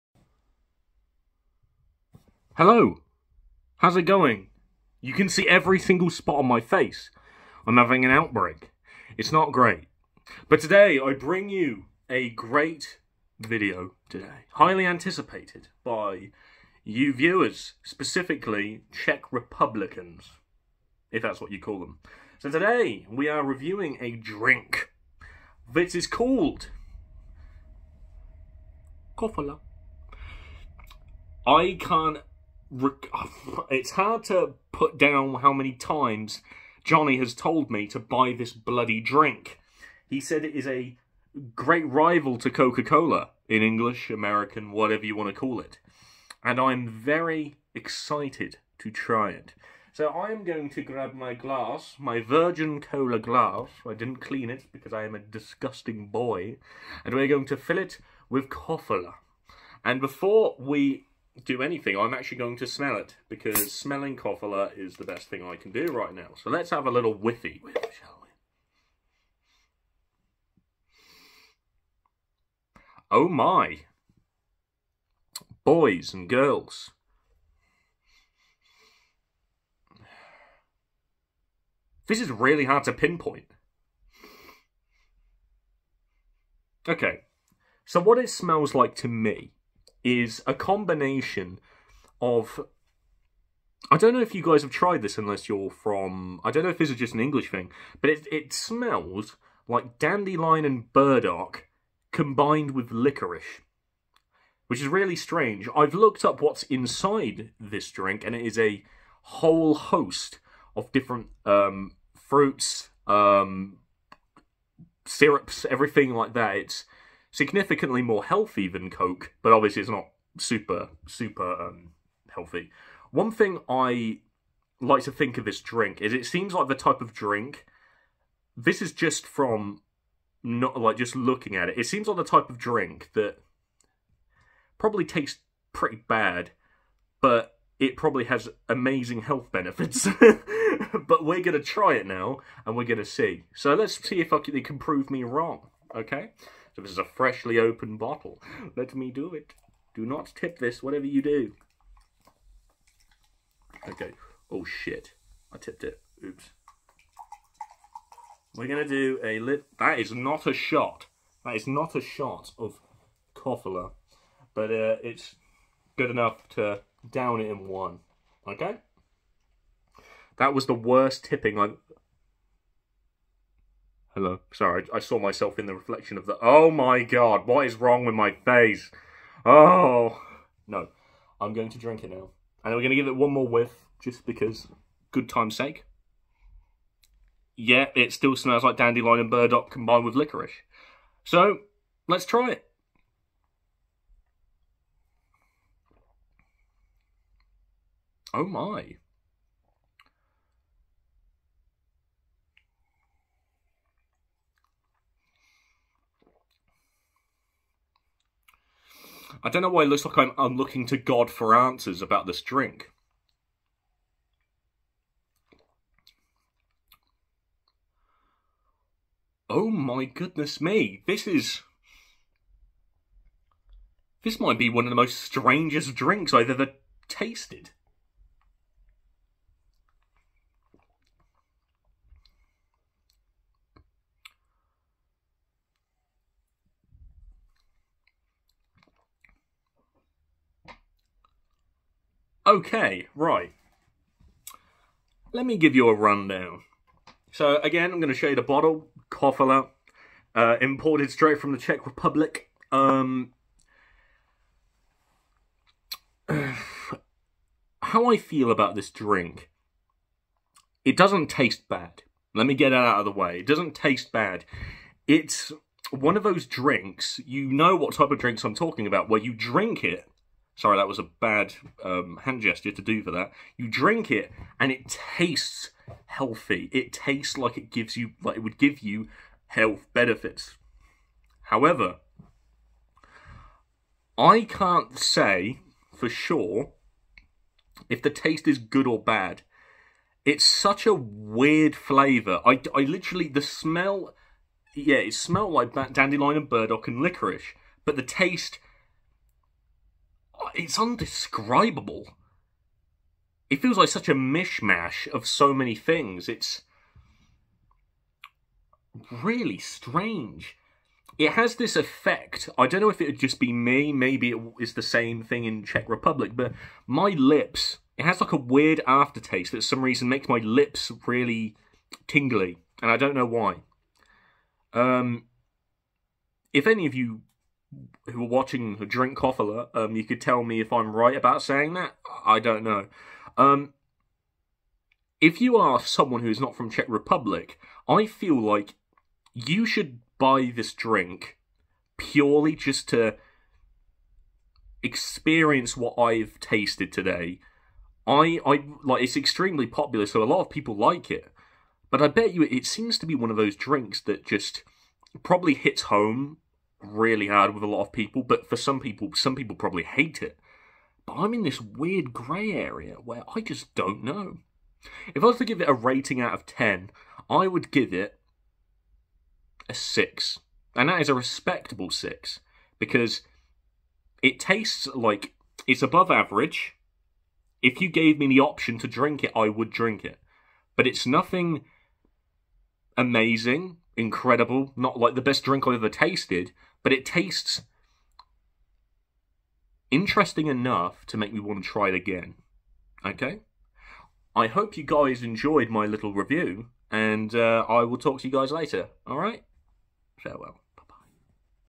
English Badger tries Kofola
After high demand from all my viewers (@oldschooljohnycz9453) I am trying Kofola! A Coke rival from Czech Republic.